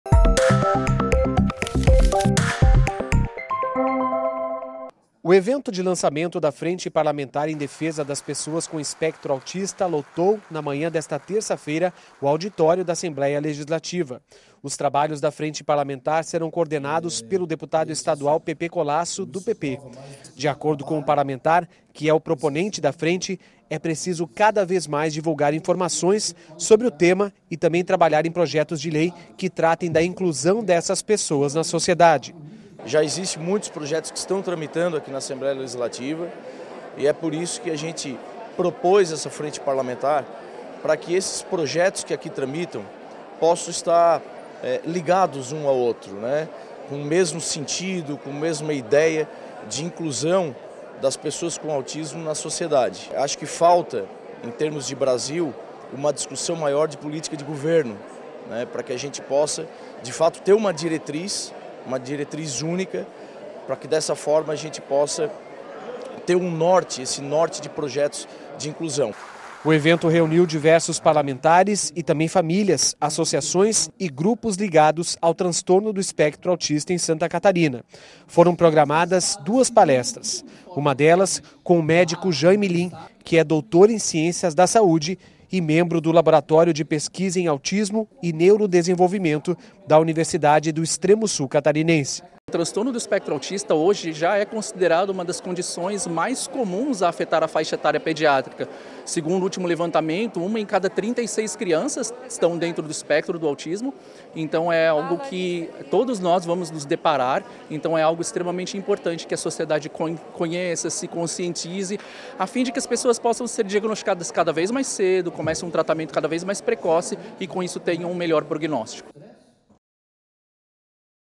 É Notícia - Lançada Frente Parlamentar em Defesa das Pessoas com Espectro Autista - 04/04/23
Em evento no Auditório Antonieta de Barros, a Assembleia Legislativa de Santa Catarina lançou, oficialmente, a Frente Parlamentar em Defesa das Pessoas com Espectro Autista, na manhã desta terça-feira (04/04). Os trabalhos da Frente Parlamentar serão coordenados pelo Deputado Estadual Pepê Collaço, do PP.